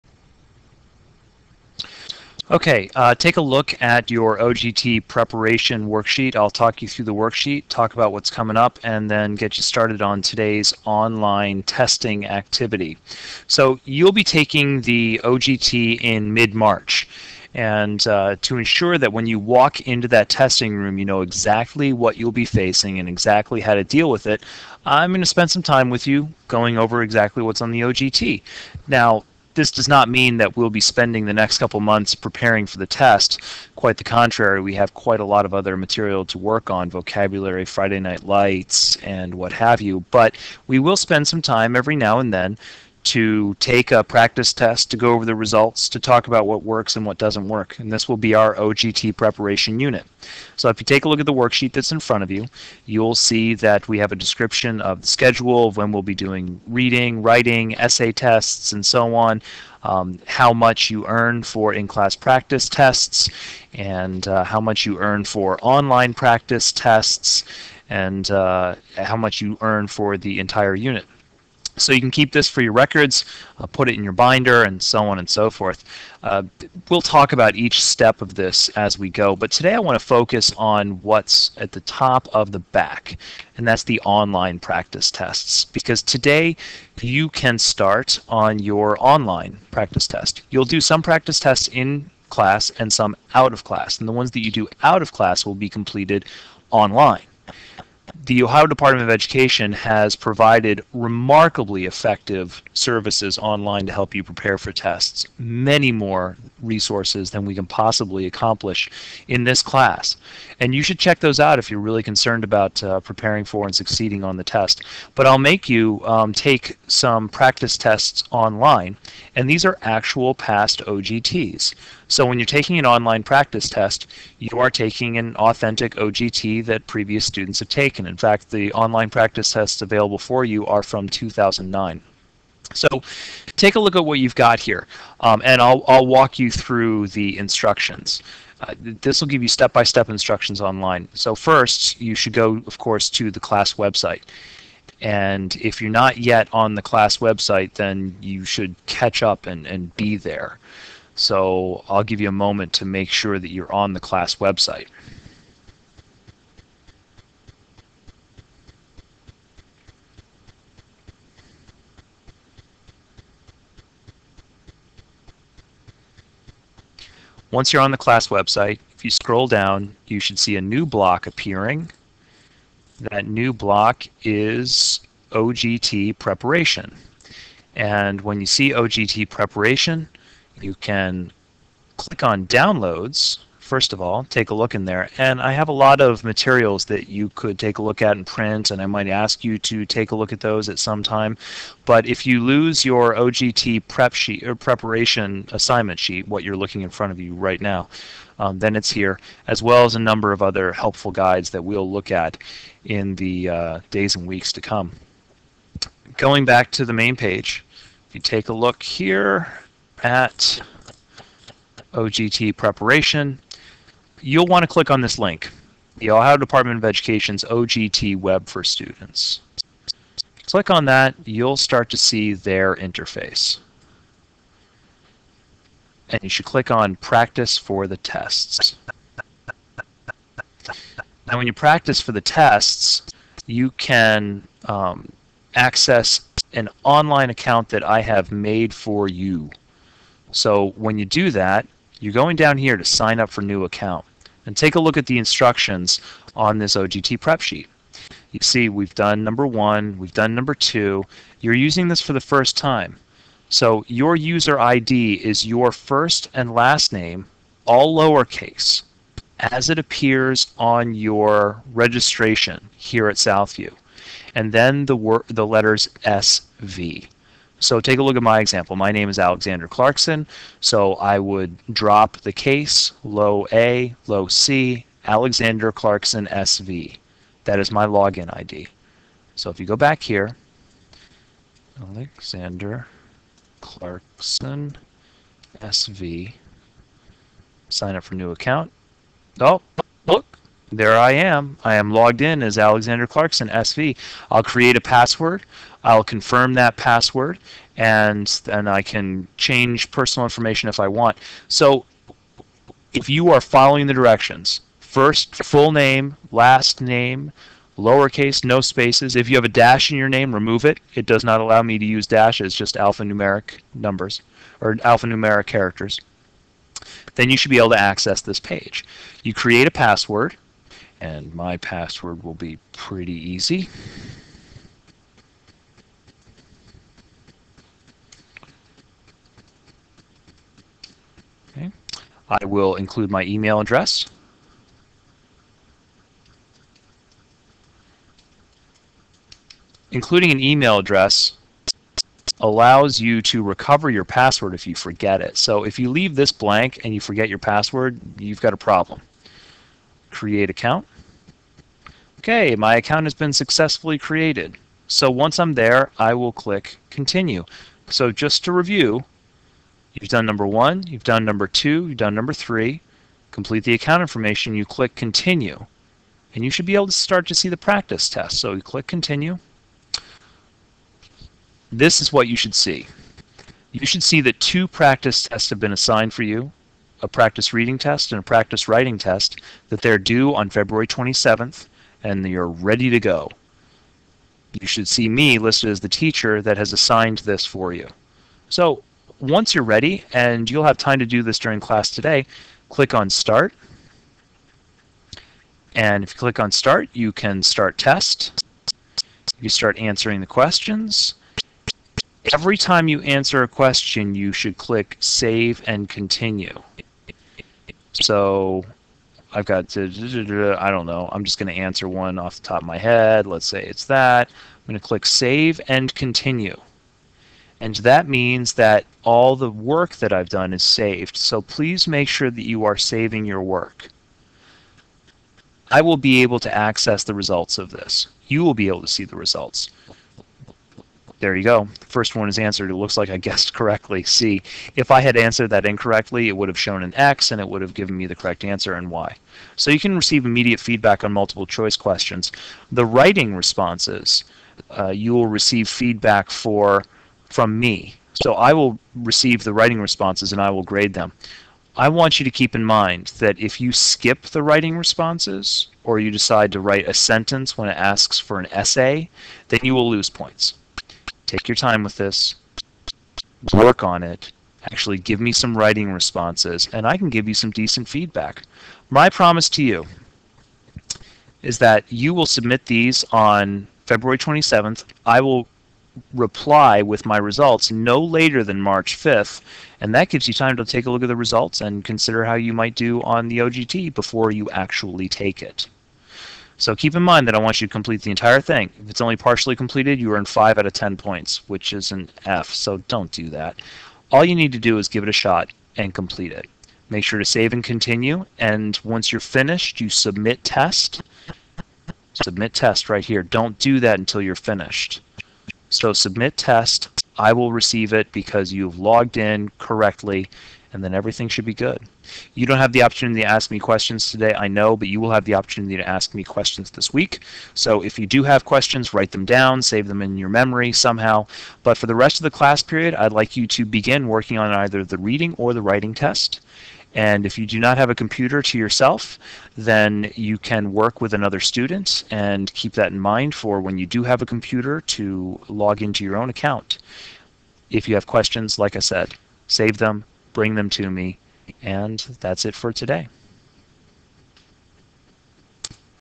OGT Preparation Introduction.wmv
This short introduction helps students log onto and access the Ohio Department of Education's on-line OGT practice.